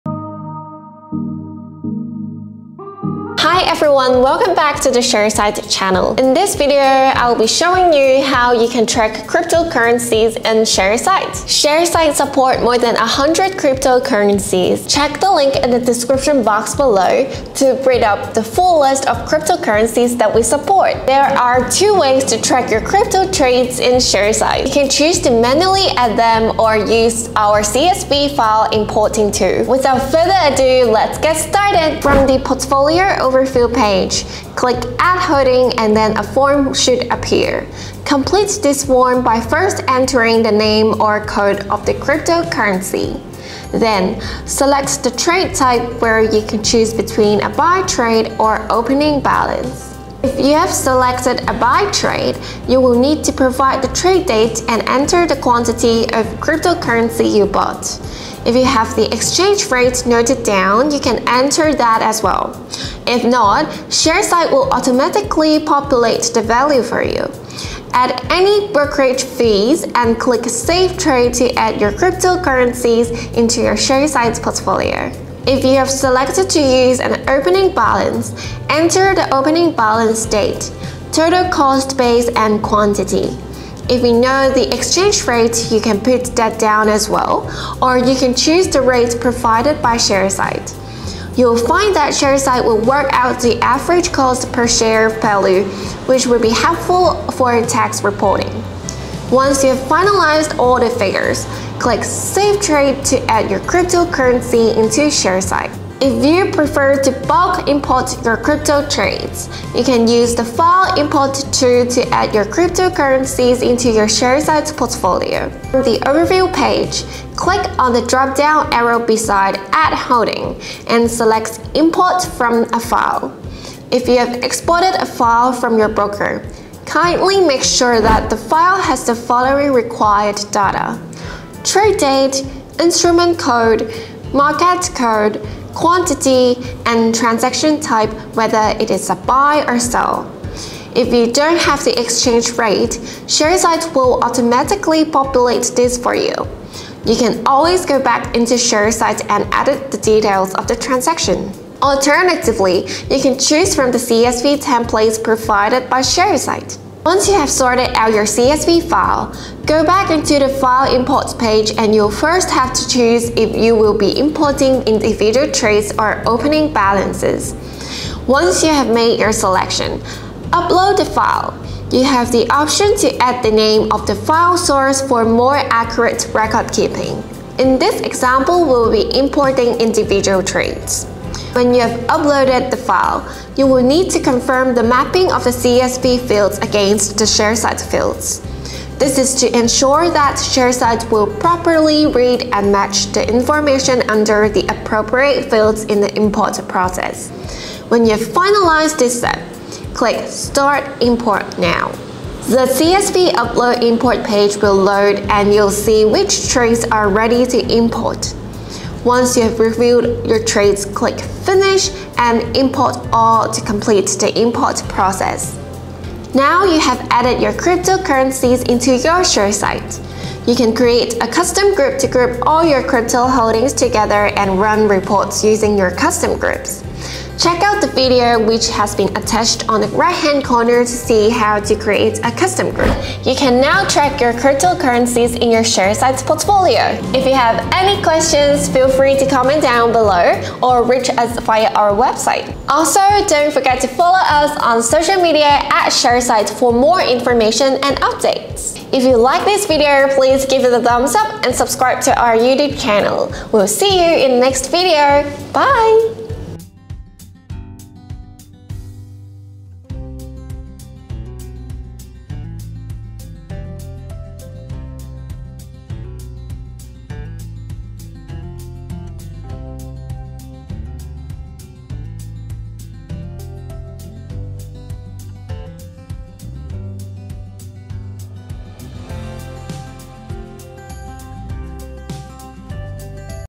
Sharesight - How to track cryptocurrencies with Sharesight
Learn how you can effortlessly monitor and track cryptocurrencies using Sharesight.

Sharesight provides support for more than 100 different cryptocurrencies (full list noted on Sharesight's help page below).

0:00 - Intro
0:33 - Portfolio overview page
0:39 - Manual holding
2:00 - Import from a file
3:45 - Custom groups
3:53 - Closing
4:17 - Outro

Powerful portfolio tracking software that lets you check your investments in one place with award-winning performance, dividend tracking and tax reporting.